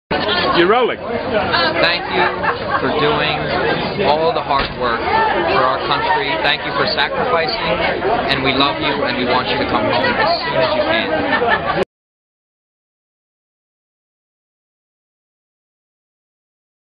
Winkball's Faces for the Forces at Tea and Sympathy New York 2009
Goodwill messages from New York City! Collected by WinkBall and the Tea And Sympathy team. Much love and merry Christmas to all!